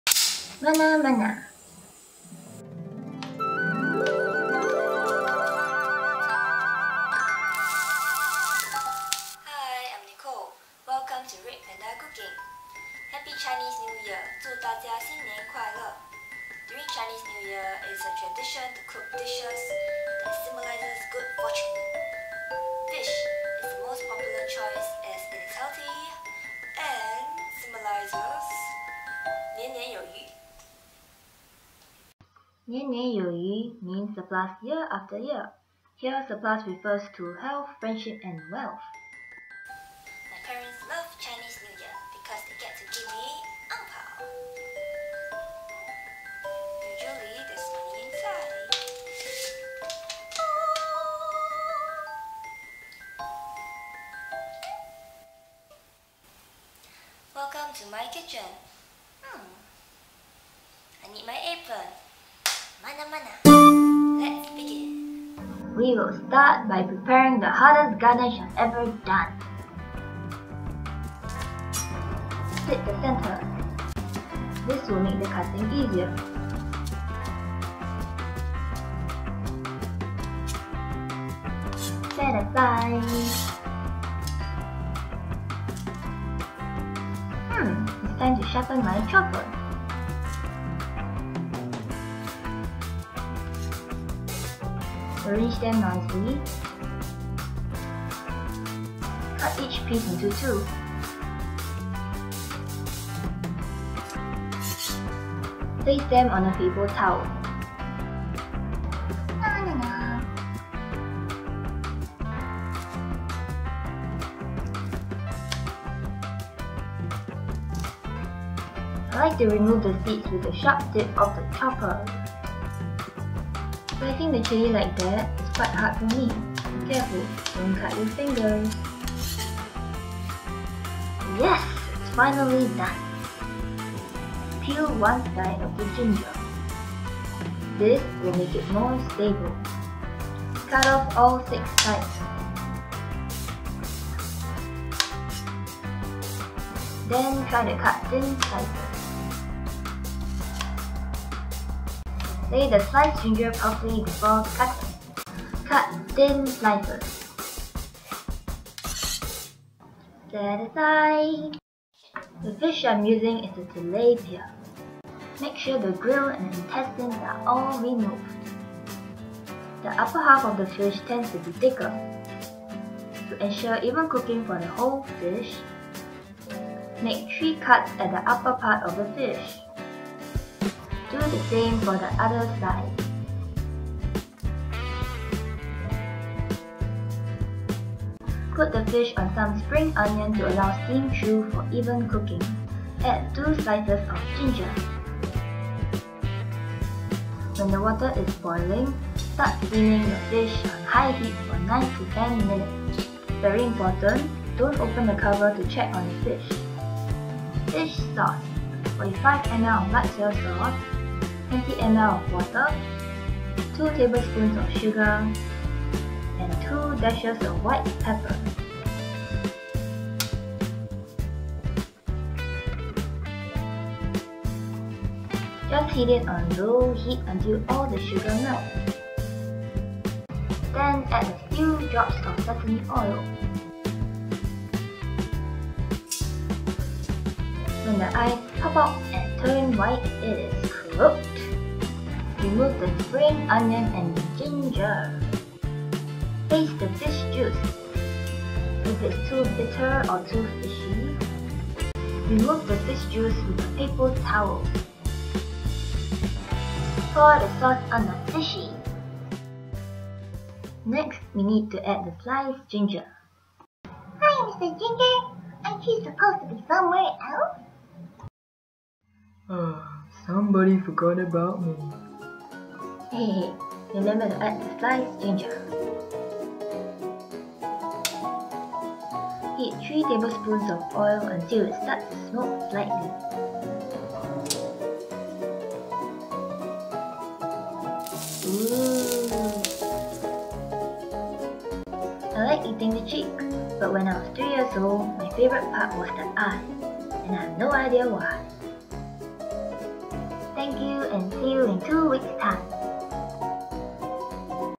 Chinese Steamed Fish * 中式蒸魚 *
Chinese Steamed Fish is a healthy and popular dish during Chinese New Year.
Ingredients:
Whole steam fish ( Tilapia or sea bass etc.)
Spring onion
ginger
chili

Fish sauce:
light soy sauce 45 ml
water 20ml
2 tablespoon of sugar
2 dash of white pepper
a few drops of sesame oil
3 tablespoon of oil

主料：
一整条魚（尼罗红或鱸魚等）
蔥
姜
辣椒

魚露：
淡醬油45毫升
水20毫升l
2湯匙糖
少许白胡椒
麻油数滴
3湯匙油